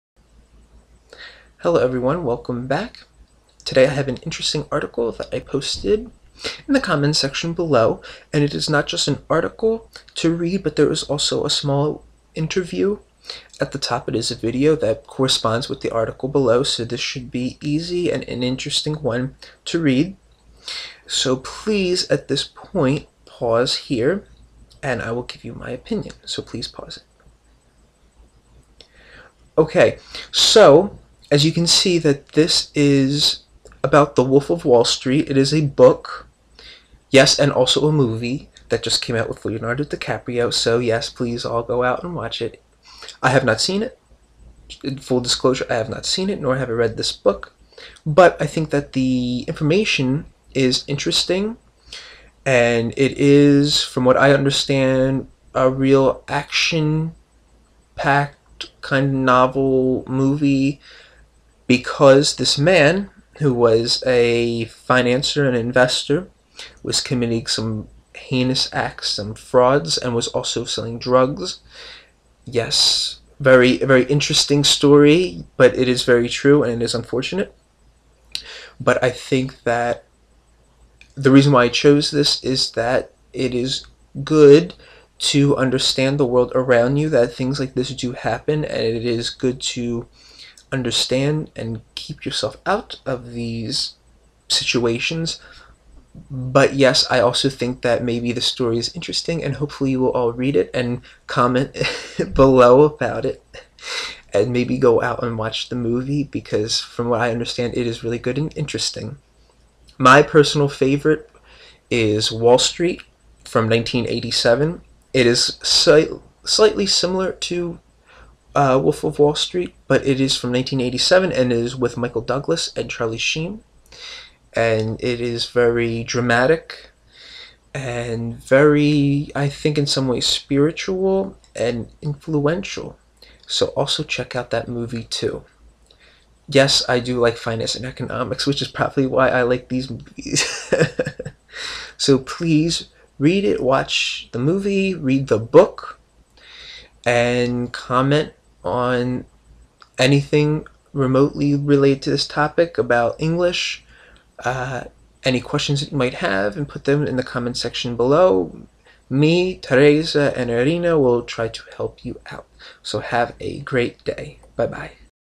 The Wolf of Wallstreet
Jordan Belfort on seeing "The Wolf of Wall Street": "I felt myself sweating at certain points"

On the heels of Thursday's 2014 Oscar nominations announcement, which revealed "The Wolf of Wall Street" being included among the films fighting for the Best Picture win, on Monday evening Piers Morgan invited Jordan Belfort to join him exclusively for a face to face interview.